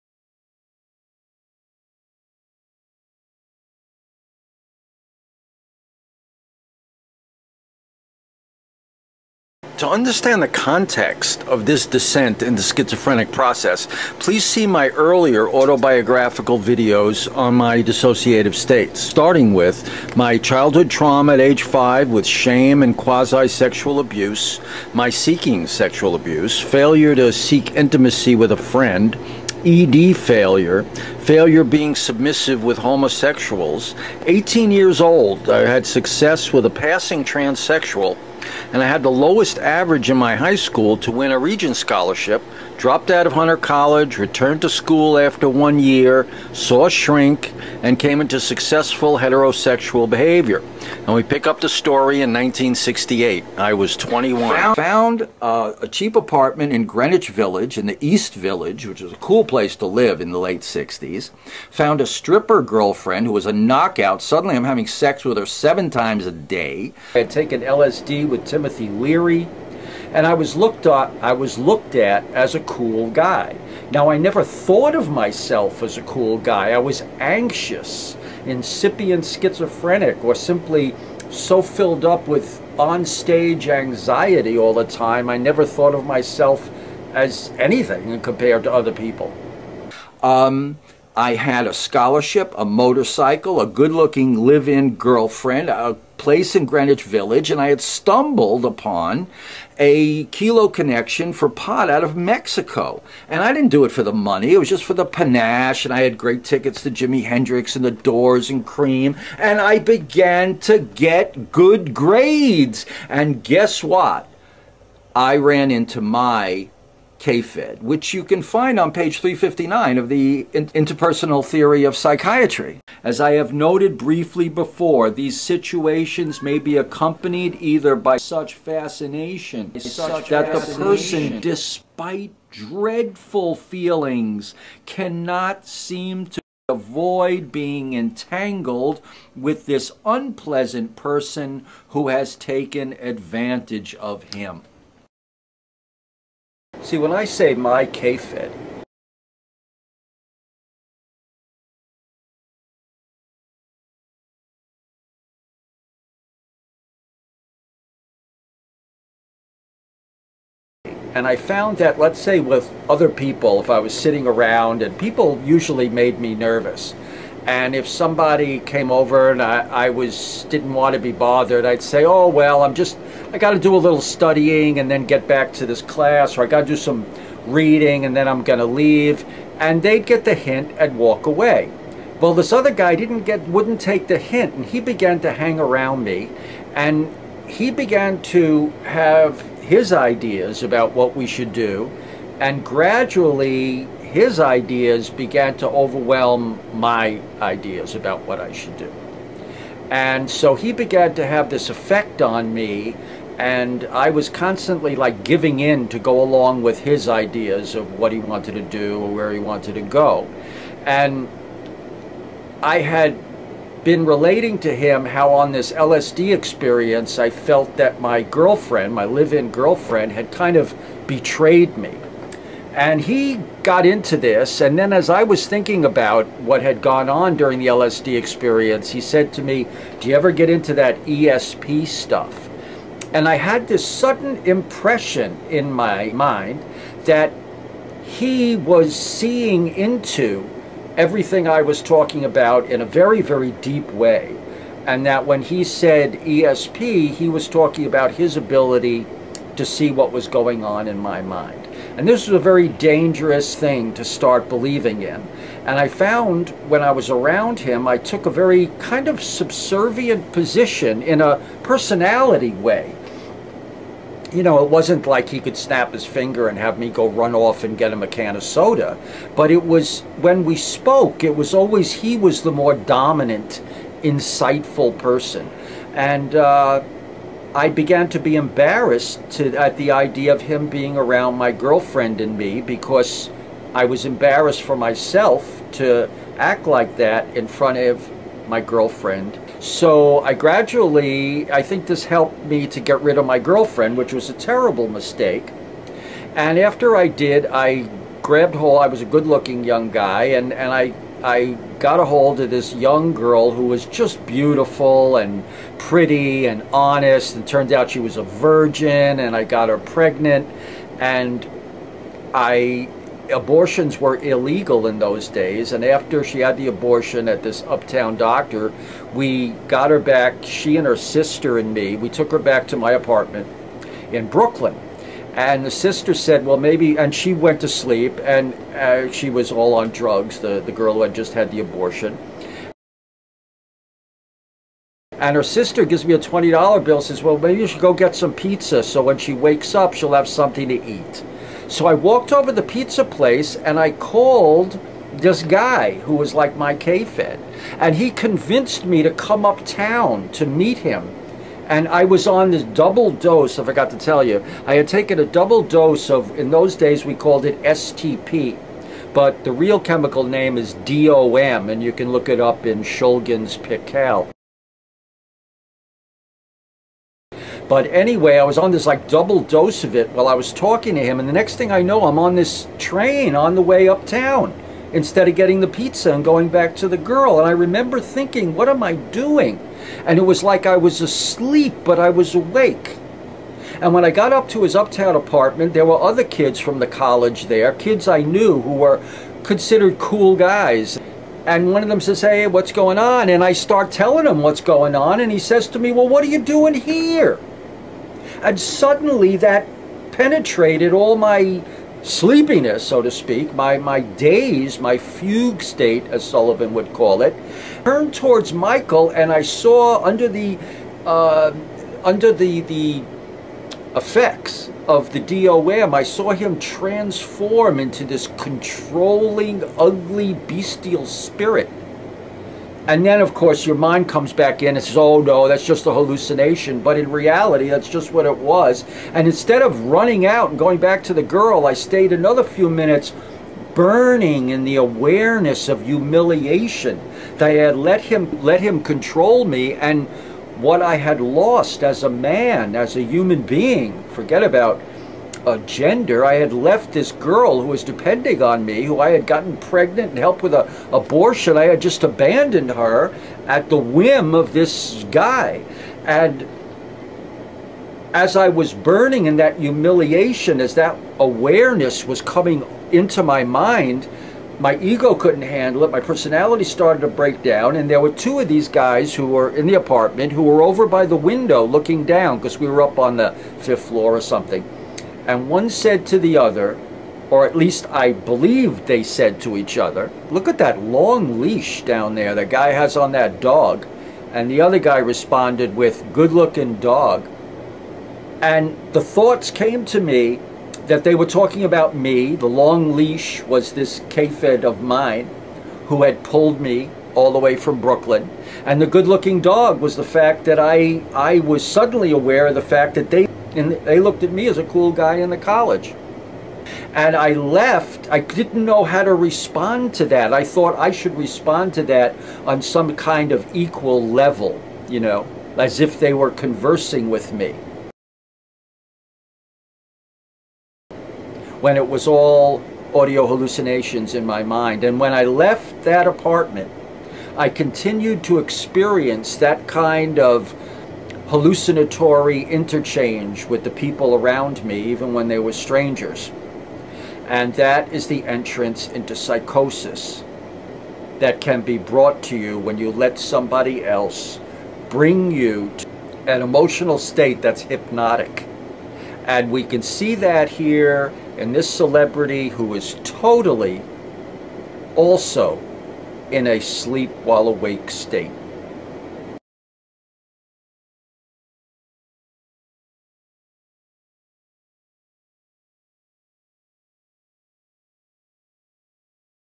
Entrance into Schizophrenic Process
Altho the beginnings towards this state started when i was 5 - and see my videos on these -  this demonstrates the Sullivanian doctrine found in "The Interpersonal Theory of Psychiatry": By allowing someone unpleasant who gains security by vassalizing others to "take over" one's personality, entrance into psychosis is possible.